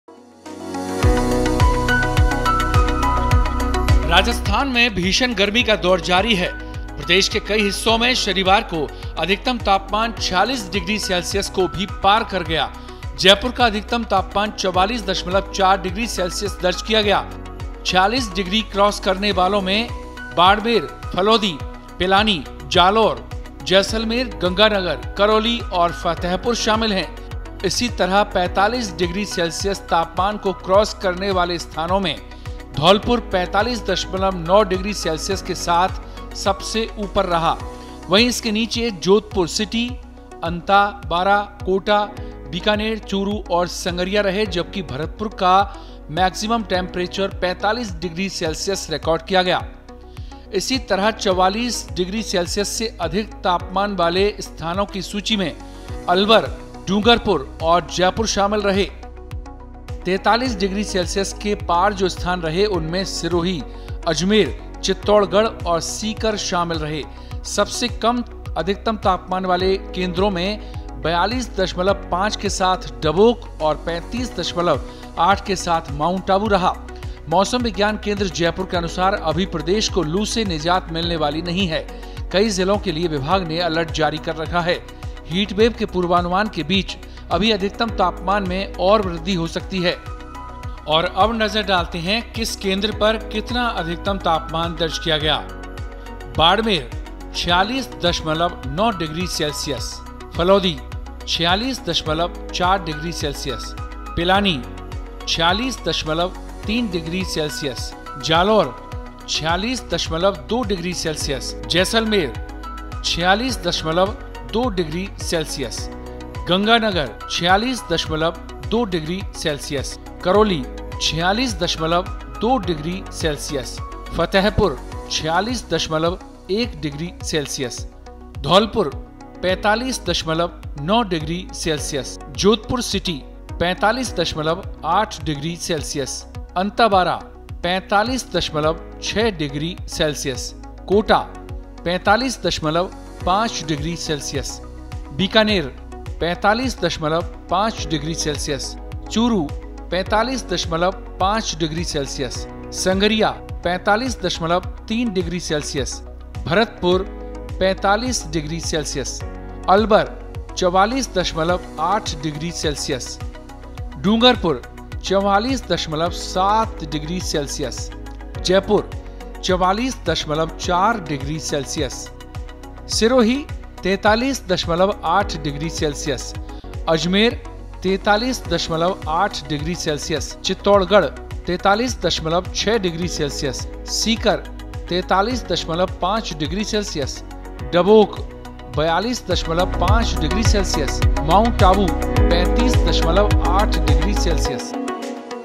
Weather Breaking News: 47 के करीब पहुंचा तापमान! मौसम विभाग का Heatwave Alert | Weather Update
Weather Breaking News: 47 के करीब पहुंचा तापमान! मौसम विभाग का Heatwave Alert | Weather Update | Latest News

Rajasthan में भीषण गर्मी का दौर जारी है। प्रदेश के कई हिस्सों में शनिवार को maximum temperature 46 डिग्री सेल्सियस को भी पार कर गया। jaipur का अधिकतम तापमान 44.4  डिग्री सेल्सियस दर्ज किया गया है। 46 डिग्री क्रॉस करने वालों में Barmer,Phalodi,Pilani, जालौर, जैसलमेर, गंगानगर, करौली  और फतेहपुर शामिल हैं। इसी तरह 45 डिग्री सेल्सियस तापमान को क्राॉस करने वाले स्थानों में  45.9 के साथ dholpur  सबसे ऊपर रहा वहीं इसके नीचे  jodhpur city, अंता/बारा, कोटा bikaner, चूरू, संगरिया रहे जबकि bharatpur का मैक्सिमम टेंप्ररेचर 45 डिग्री सेल्सियस रिकॉर्ड किया गया। इसी तरह 44 से अधिक अधिकतम तापमान वाले स्थानों की सूची में अलवर,डूंगरपुर और जयपुर शामिल रहे। 43 के पार जो स्थान रहे उनमें sirohi, अजमेर,चित्तौड़गढ़ और sikar शामिल रहे। सबसे कम तापमान वाले केंद्रों में 42.5 के साथ डबोक और 35.8 के साथ माउंट आबू रहा। मौसम विज्ञान केंद्र mosam vigyan kendra जयपुर के अनुसार अभी प्रदेश को लू से निजात मिलने वाली नहीं है। कई जिलों के लिए विभाग ने alert जारी कर रखा है। heatwave के पूर्वानुमान के बीच अभी अधिकतम तापमान में और वृद्धि हो सकती है।

अधिकतम तापमान-
बाड़मेर 46.9
फलोदी 46.4
पिलानी 46.3
जालौर 46.02
जैसलमेर 46.02
गंगानगर 46.2
करौली 46.2
फतेहपुर 46.01
धौलपुर 45.9
जोधपुर सिटी 45.8
अंता/बारा 45.6
कोटा 45.5
बीकानेर 45.5
चूरू 45.5
संगरिया 45.3
भरतपुर 45
अलवर 44.8
डूंगरपुर 44.7
जयपुर 44.4
सिरोही 43.8
अजमेर 43.8
चित्तौड़गढ़ 43.6
सीकर 43.5
डबोक 42.5
माउंट आबू 35.8